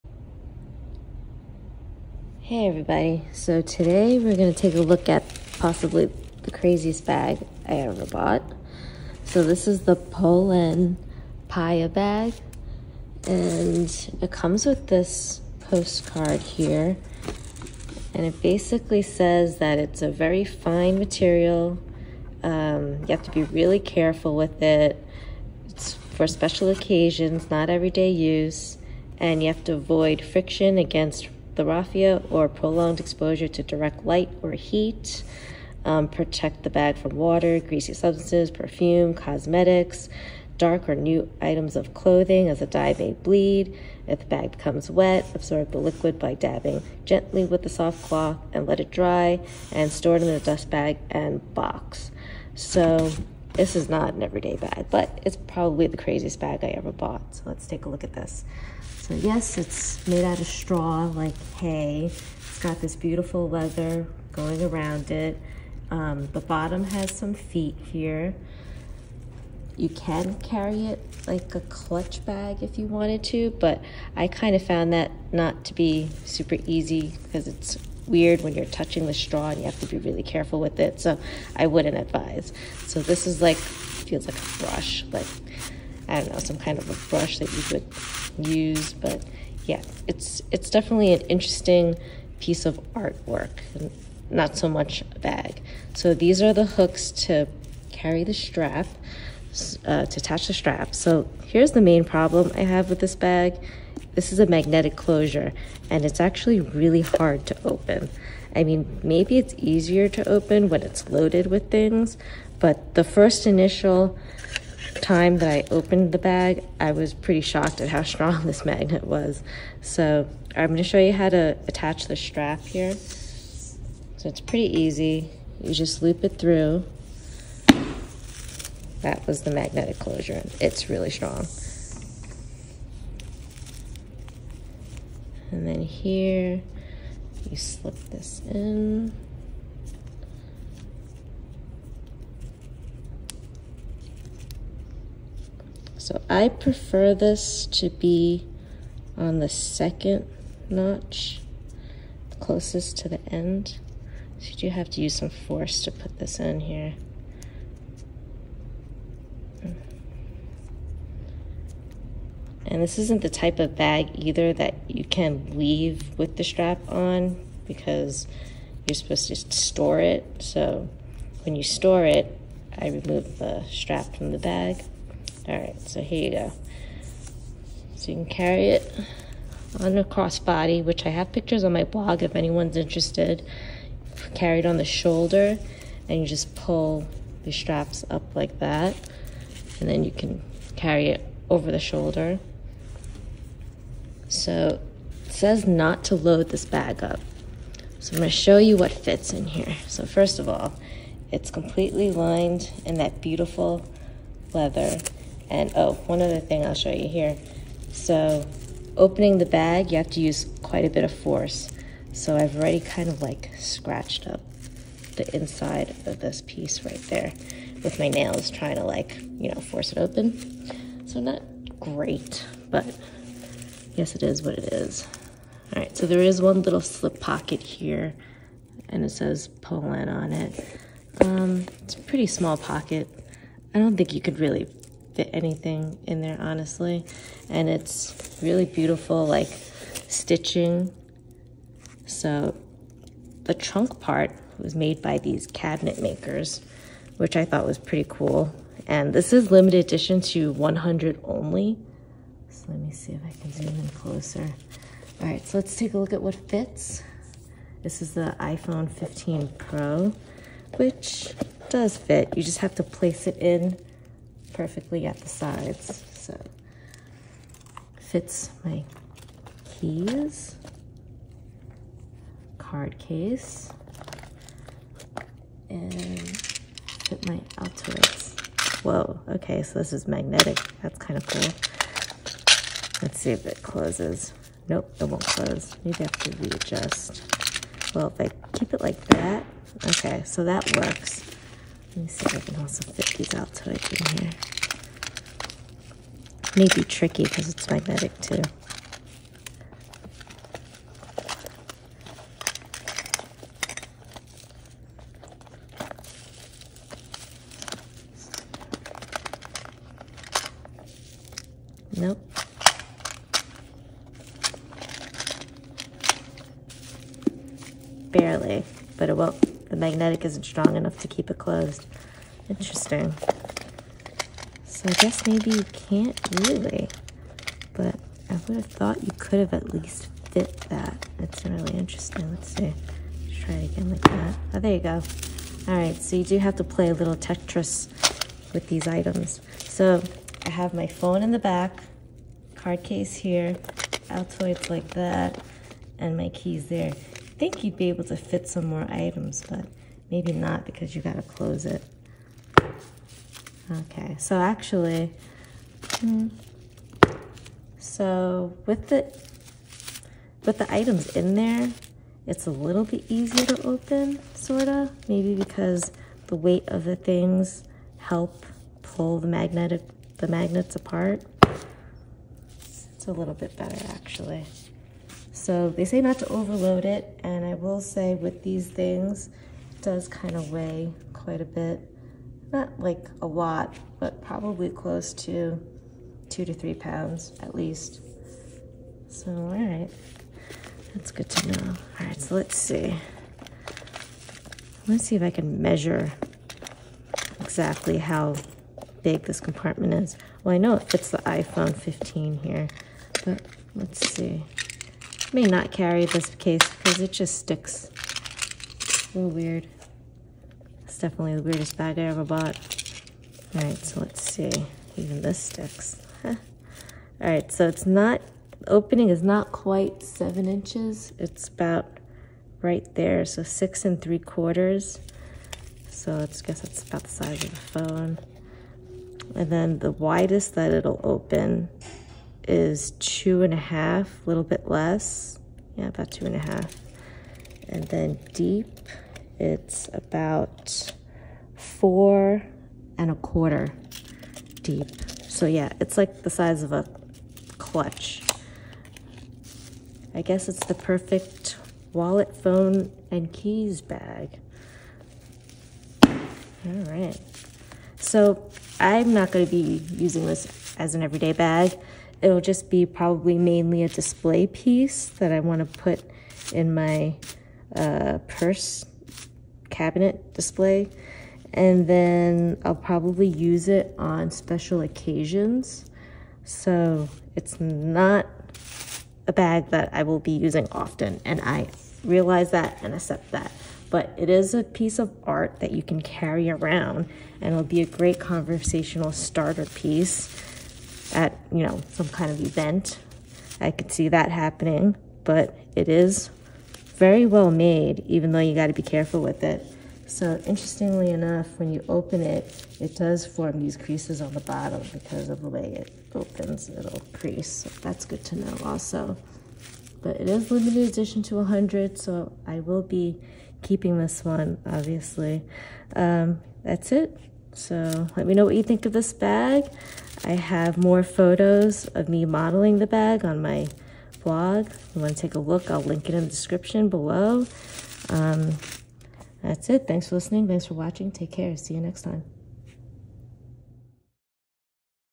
Polène Paï Bag Review (Polene Pai)
Here's my review of the Polène Paï (Polene Pai) limited edition bag. This may be the weirdest bag that I've owned to date. My husband has said to stay away from horses and cows if I'm carrying this bag.

FTC notice: This video is not sponsored or a paid advertisement.